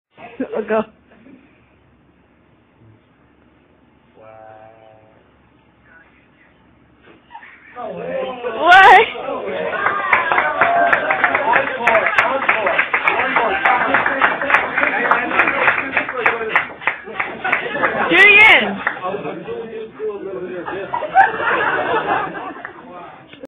tiny girl through a plastic hanger
girl through a hanger plus warners happy dance